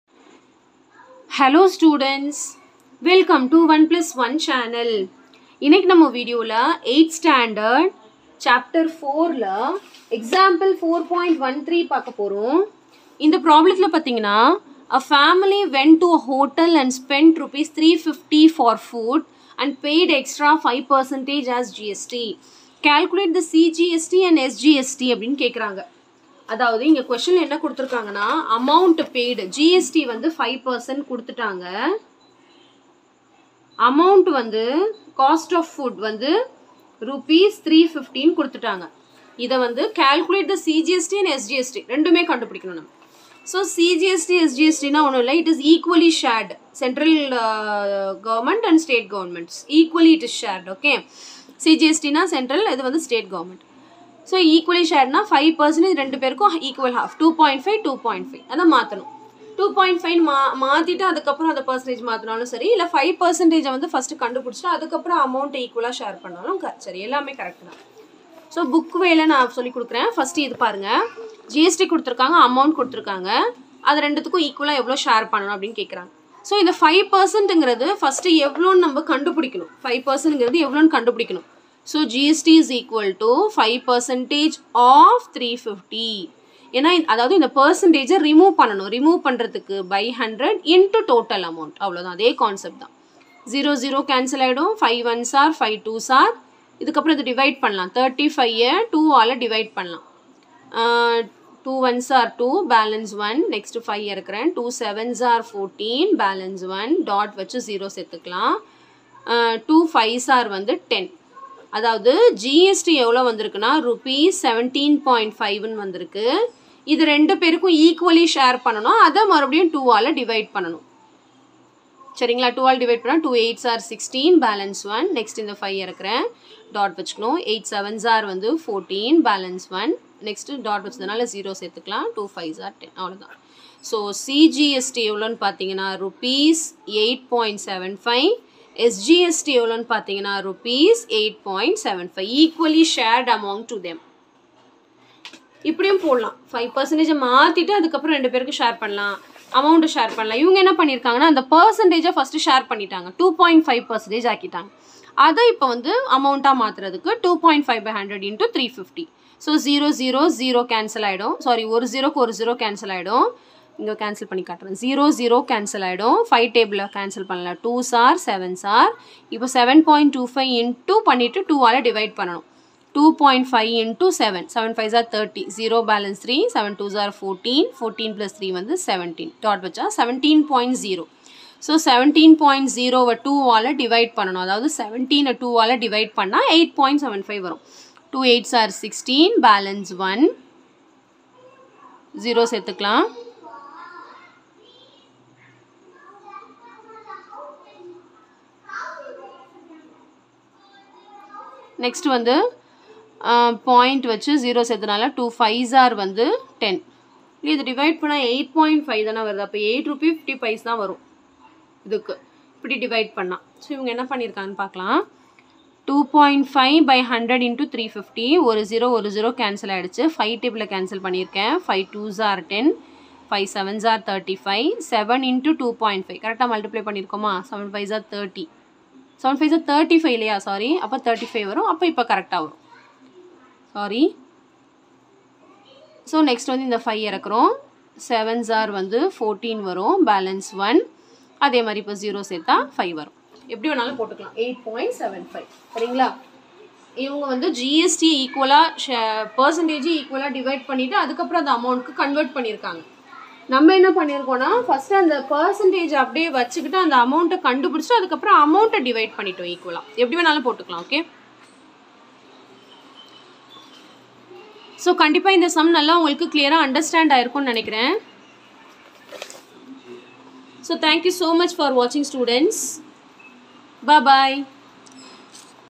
8th Maths Ch-4 Life Mathematics | Example-4.13 | Samacheer One plus One channel
This video contains for 8th Standard Maths  Chapter-4 Life Mathematics Example-4.13 complete solution.

My Another Channel 👇
Sangeetha Media